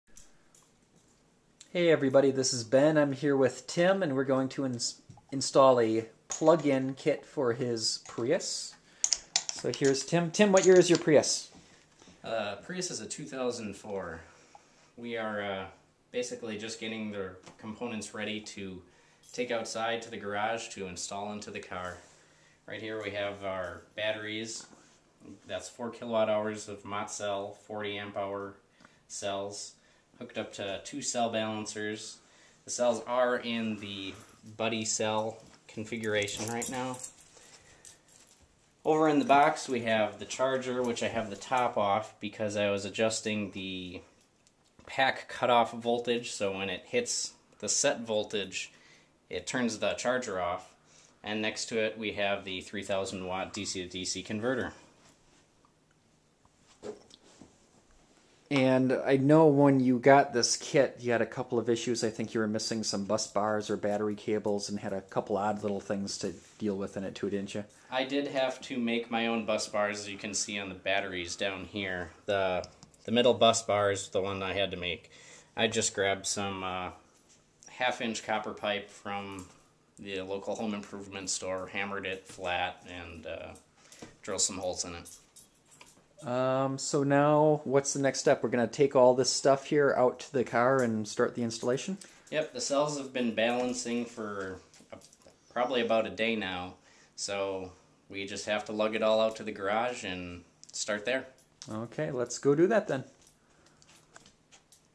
Enginer Prius Plug-in Conversion part 1
Converting a Prius to PHEV using the Enginer 4KWh kit.
Part one of several videos.